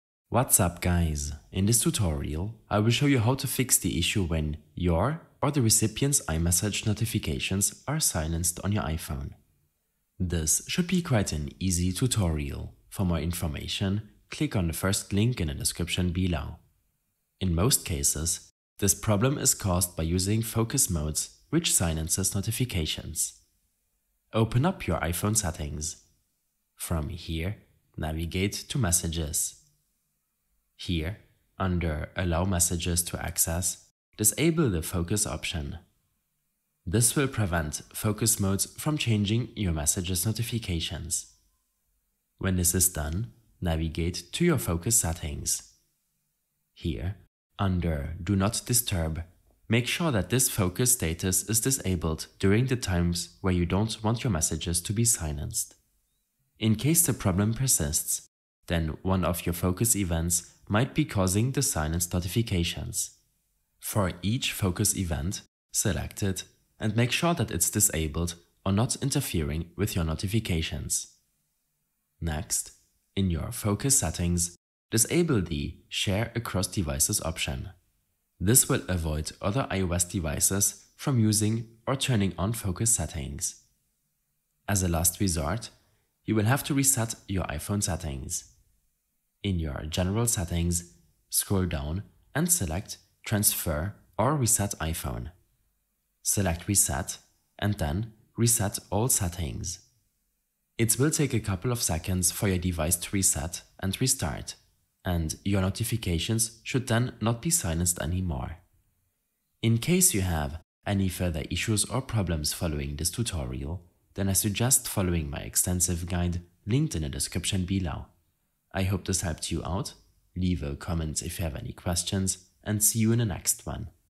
How To Fix Notifications Silenced on iPhone
A short tutorial on how to fix the issue when your or the recipient's iMessage notifications are silenced on your iPhone.

Timestamps:
0:00 - Introduction
0:17 - Disallow Messages to Access Focus
0:42 - Disable Focus Status
0:56 - Disable Focus Events
1:12 - Disable Share Across Devices
1:24 - Reset iPhone Settings